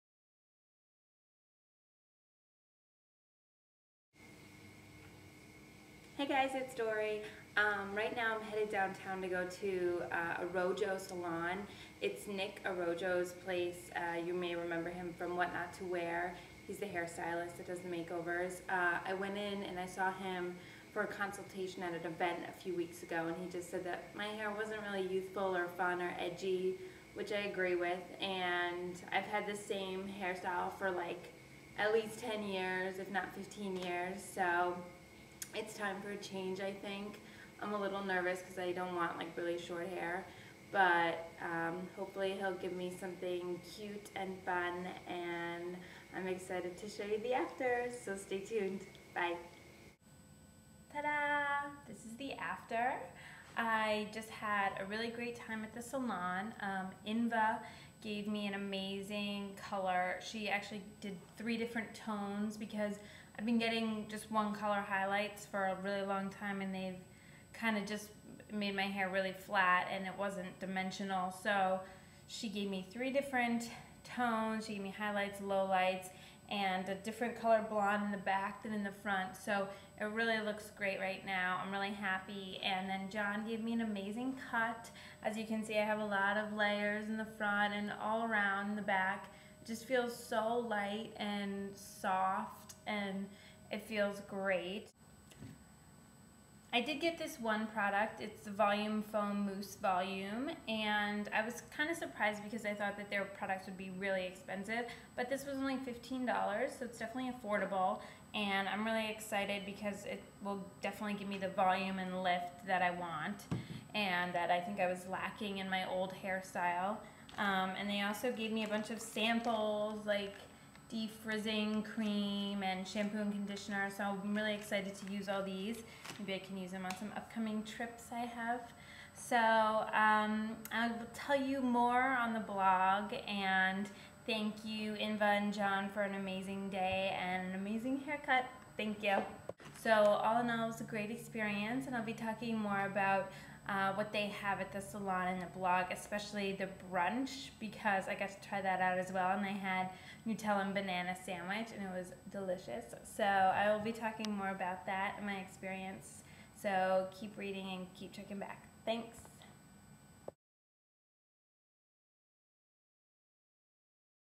Dory's New Haircut from ARROJO STUDIO!
Dory was treated to a new cut and color by Inva and John at ARROJO STUDIOS in NYC! See the before and after!